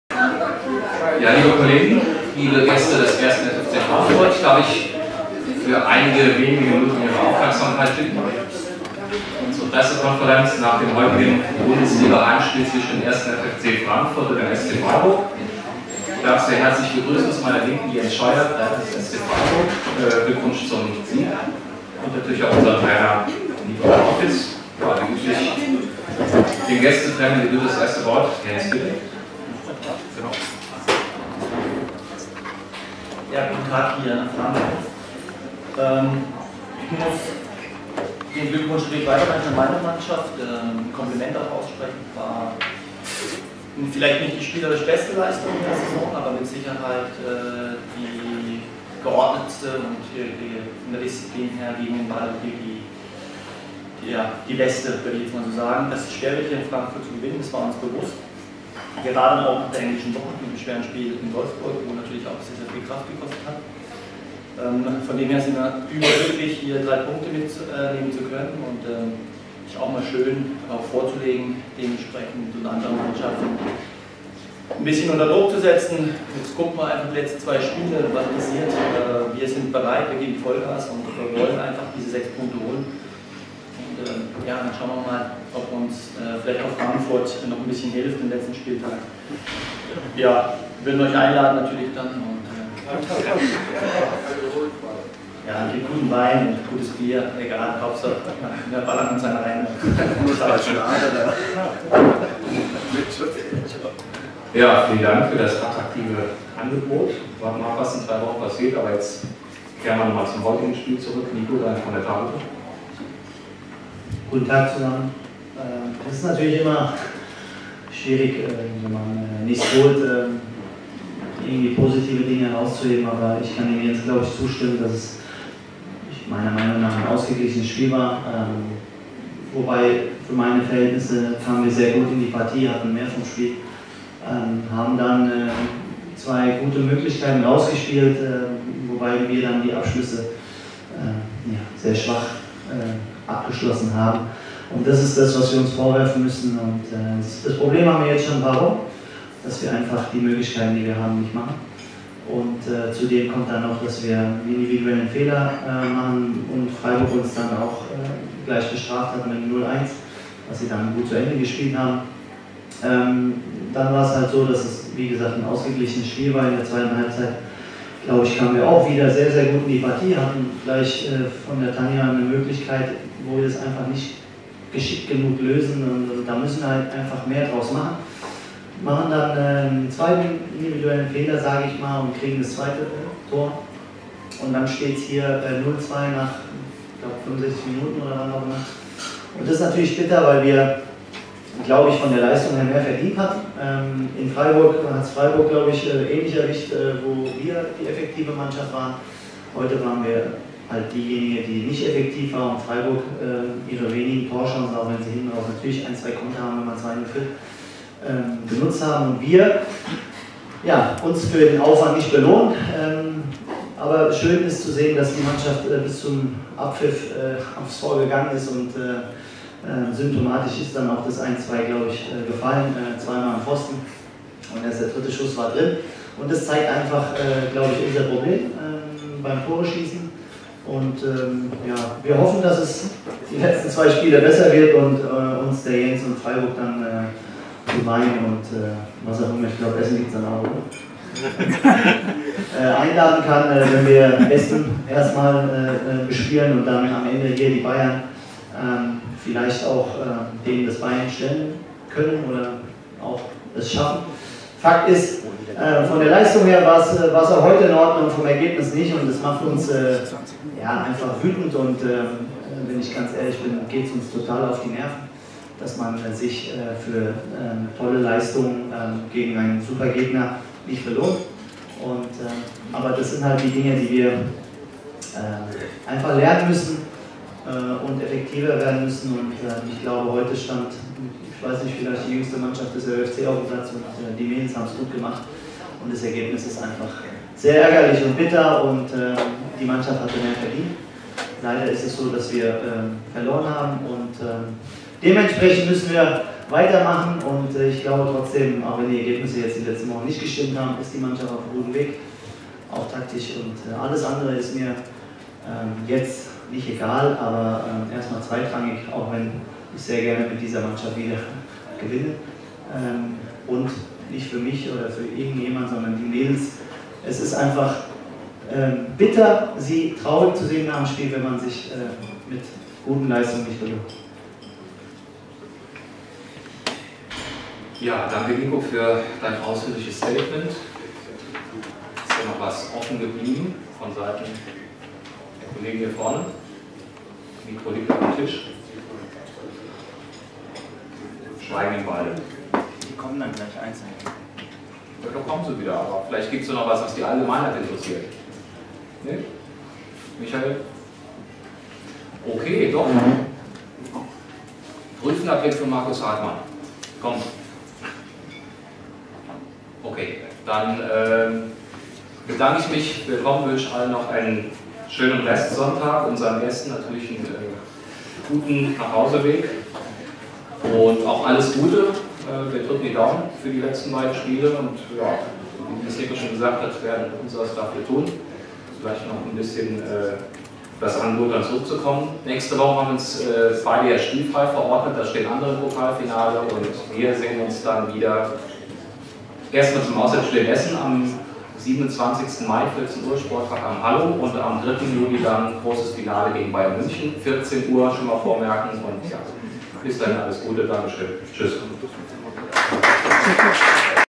Pressekonferenz 1. FFC Frankfurt - SC Freiburg 13. Mai 2018
Die Pressekonferenz nach dem Spiel zwischen dem 1. FFC Frankfurt und dem SC Freiburg in der Allianz Frauen-Bundesliga am 13. Mai 2018. Mit Niko Arnautis (Cheftrainer 1. FFC Frankfurt), Dirk Zilles (Medienreferent 1. FFC Frankfurt) und Jens Scheuer (Trainer SC Freiburg).